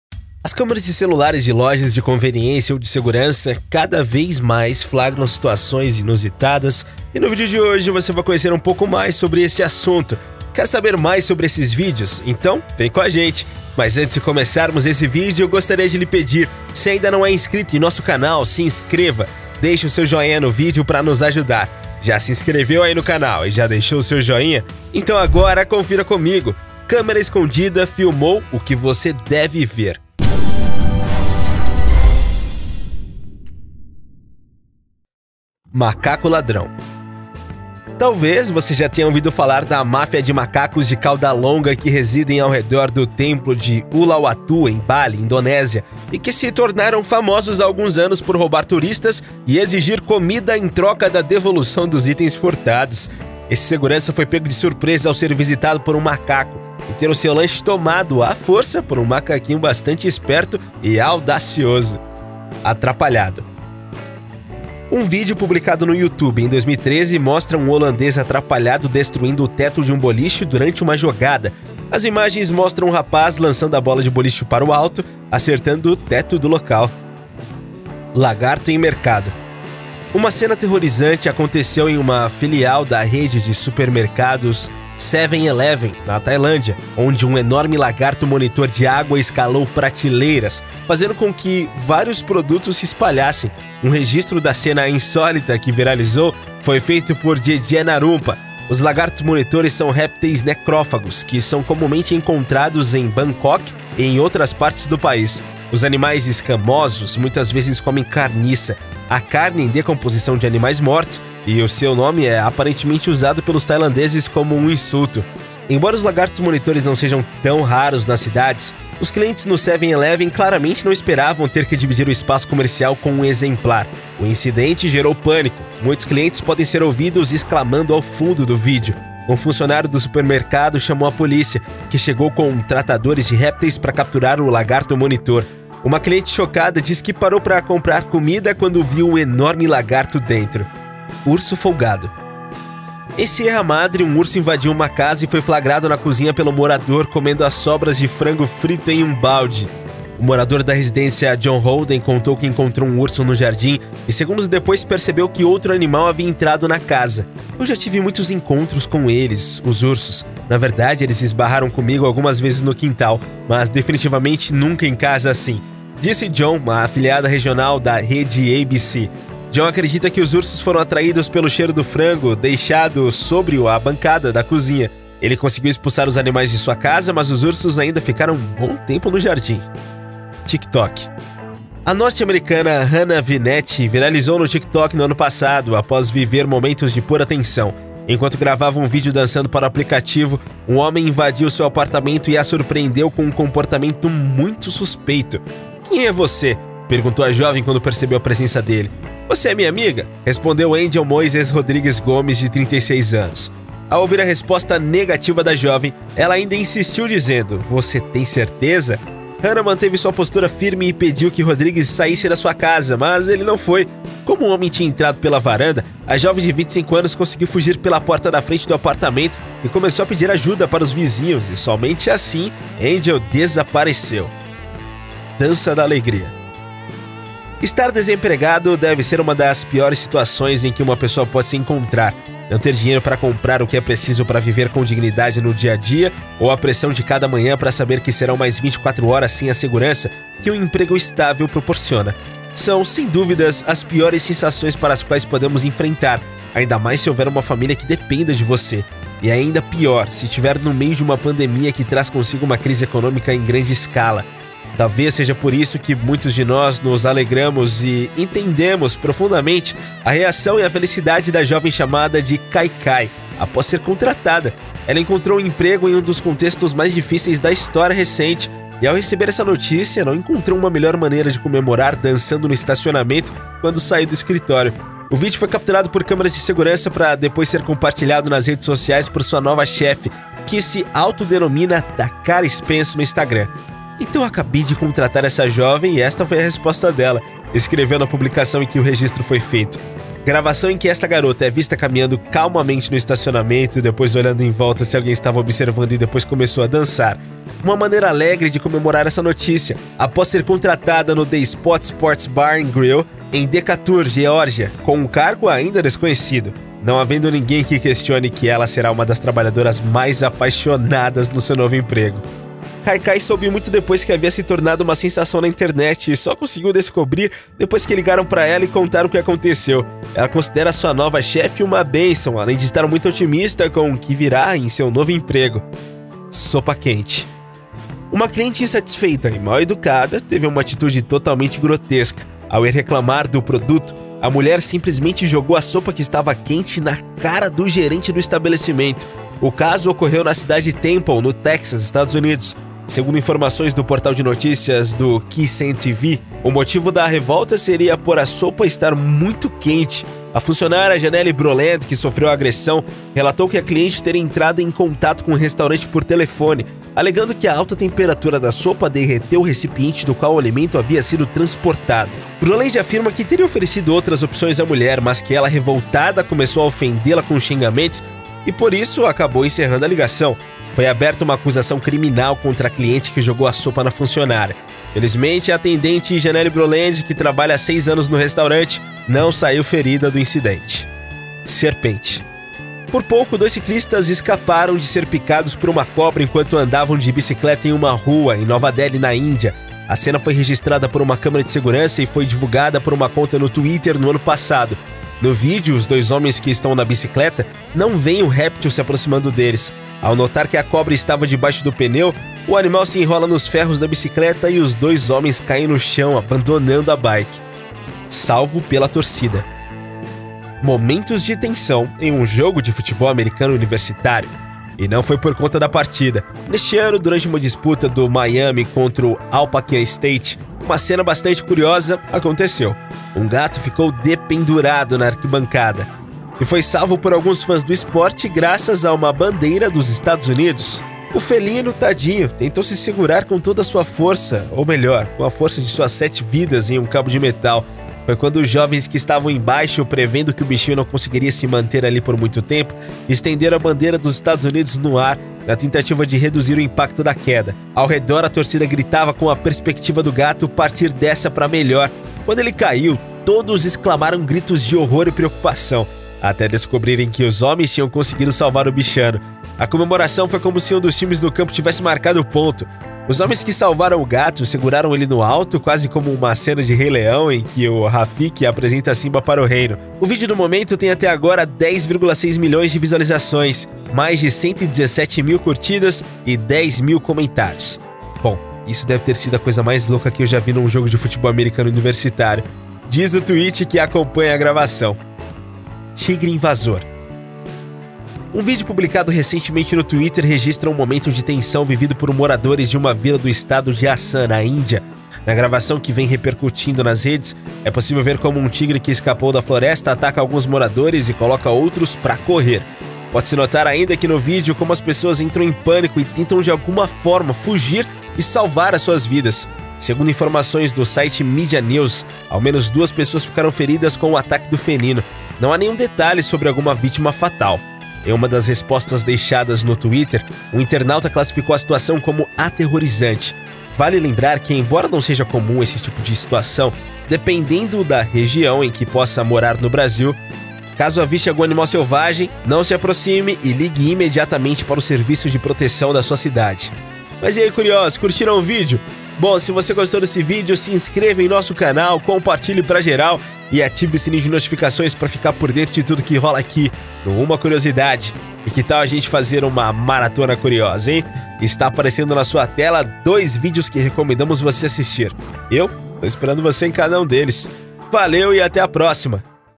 CÂMERA ESCONDIDA FILMOU O QUE VOCÊ DEVE VER
As câmaras de celulares, de lojas de conveniência ou de segurança cada vez mais, flagram situações inusitadas e no vídeo de hoje você vai conhecer um pouco mais sobre este assunto. Quer saber mais sobre estes vídeos? Então, vem com a gente.